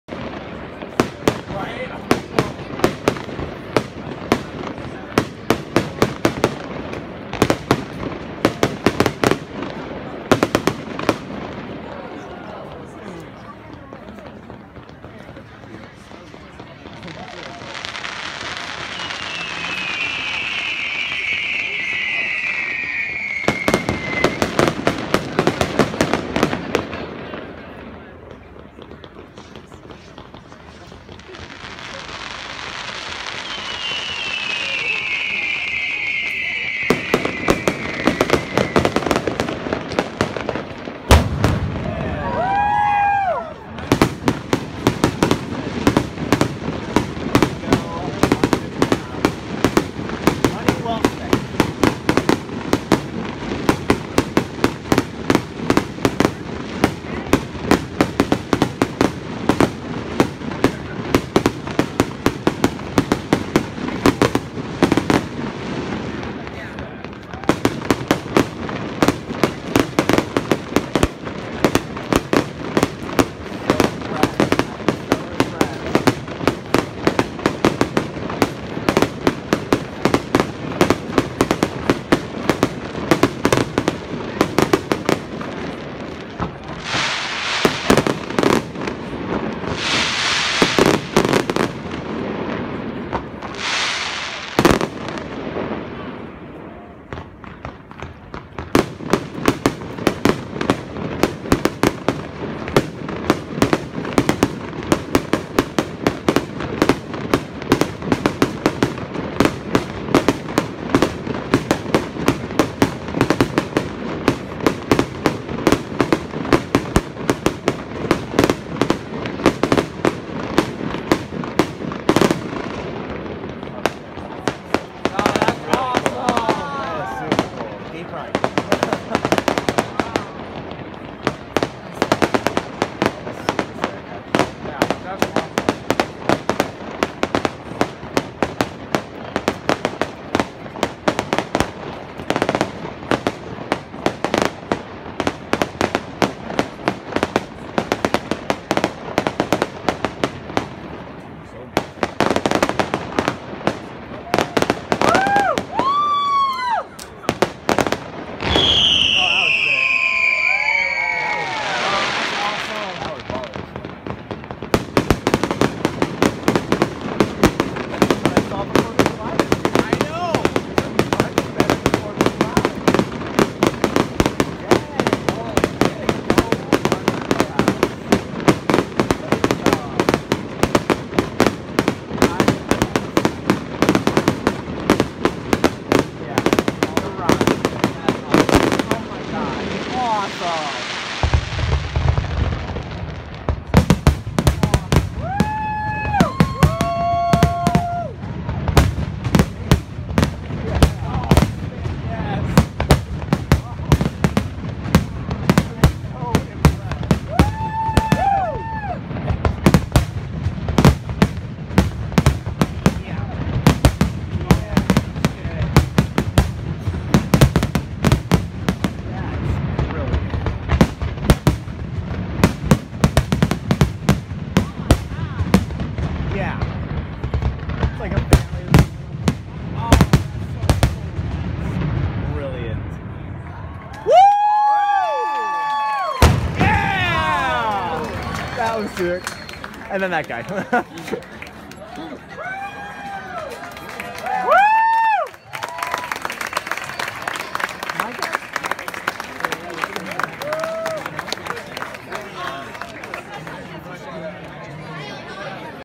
Fireworks over the Cedar River, Waterloo, IA
Fireworks over the Cedar River to celebrate the kickoff of the 100th anniversary season of the Waterloo Community Playhouse on July 8, 2016.